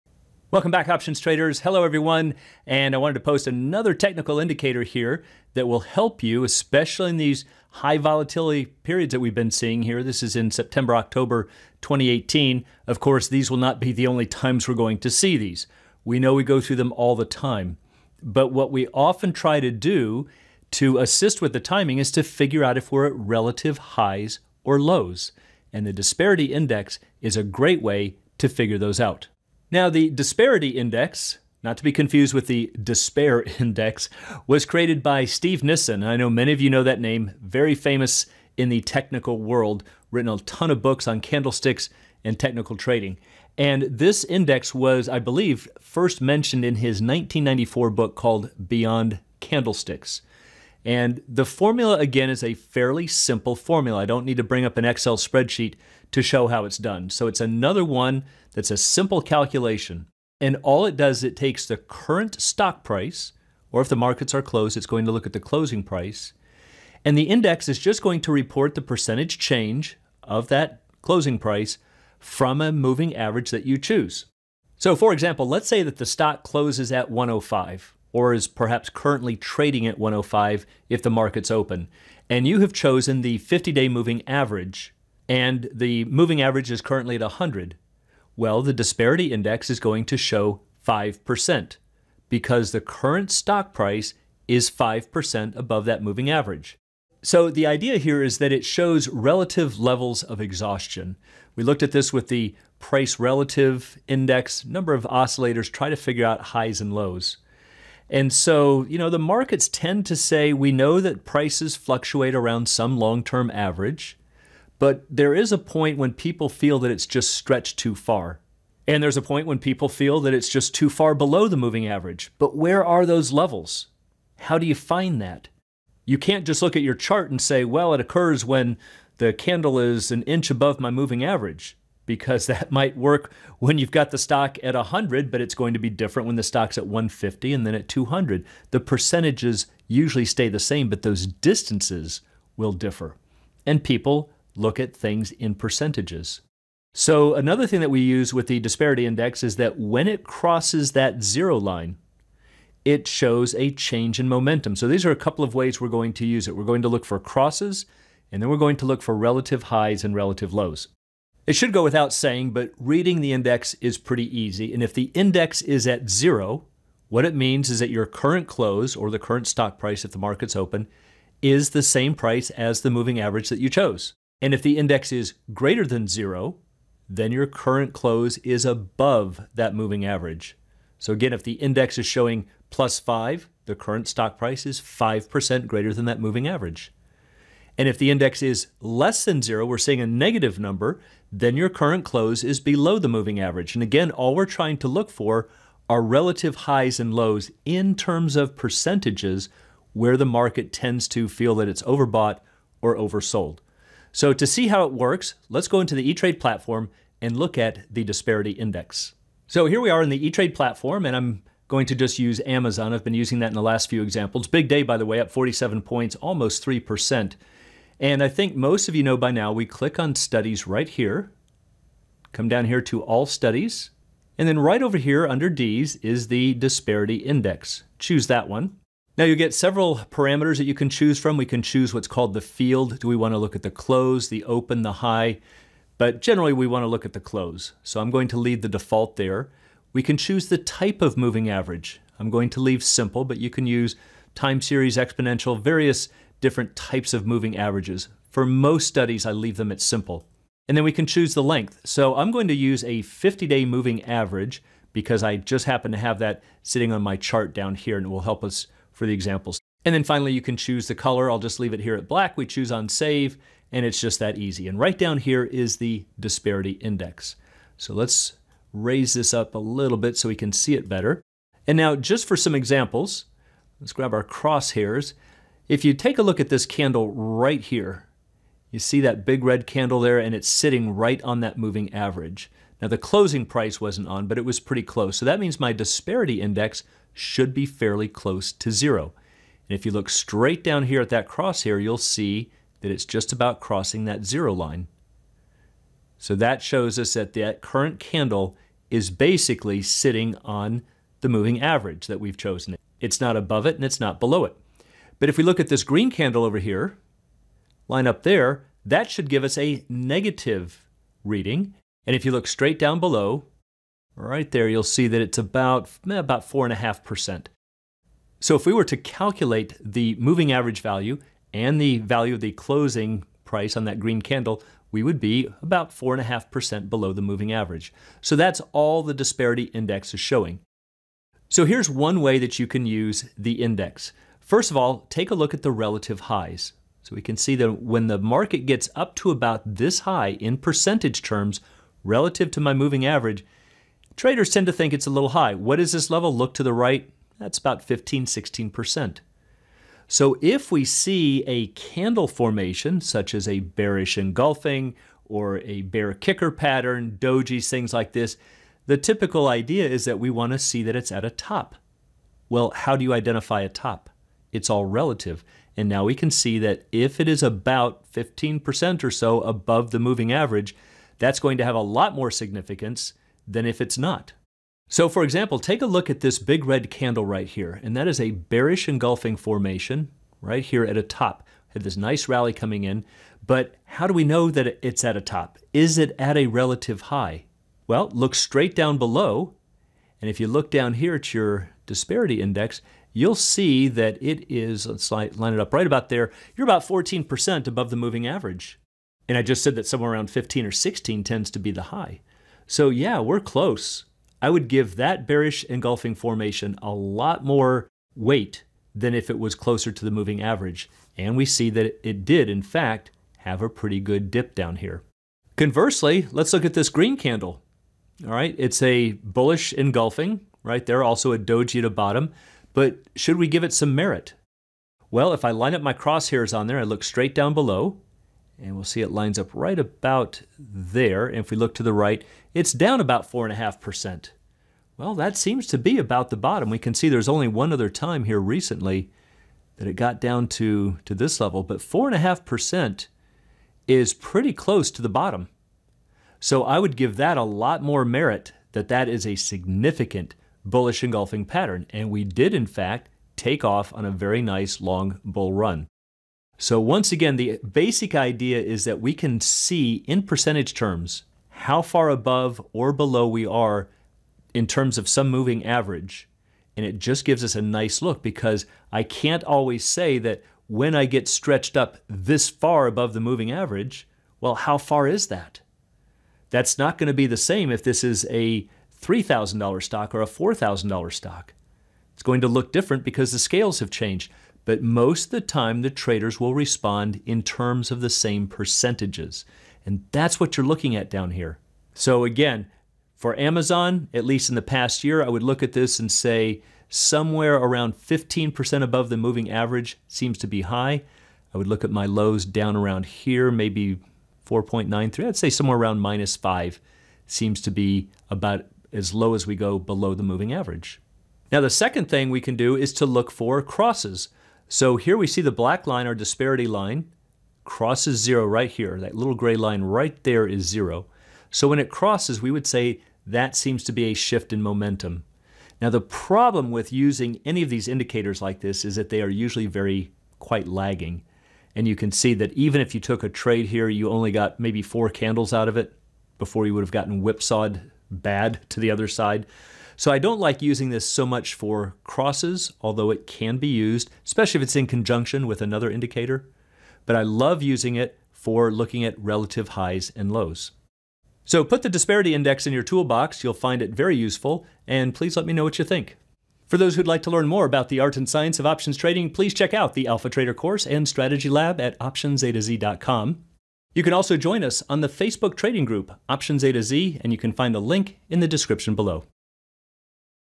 Technical Analysis: Disparity Index
How do you tell if your stock's relatively high or low? Stock prices follow a long-term moving average, but they have highs and lows around it. The Disparity Index shows just how high or low you are from a moving average. It's a great tool for entering or hedging options positions.